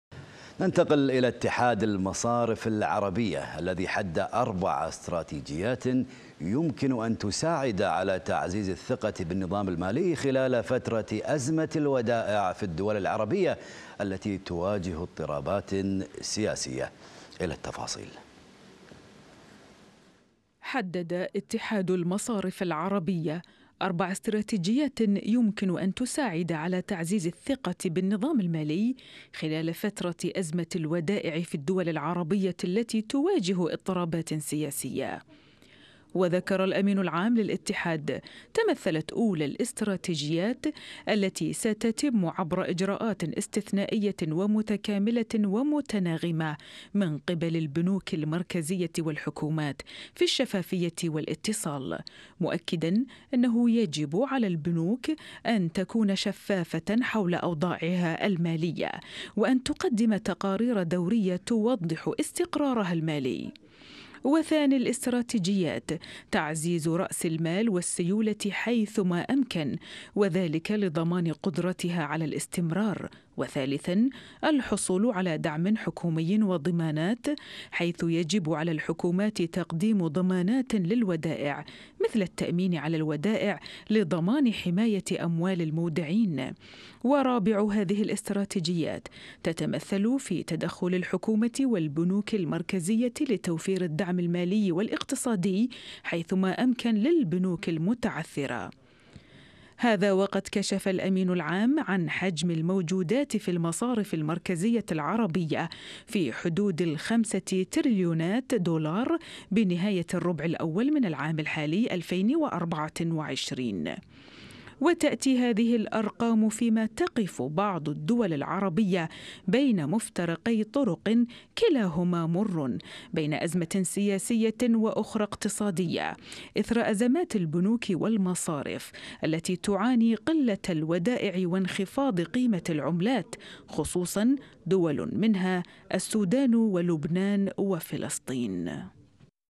4 استراتيجيات لتعزيز الثقة في النظام المالي العربي في مواجهة أزمة الودائع
4 استراتيجيات لتعزيز الثقة في النظام المالي العربي في مواجهة أزمة الودائع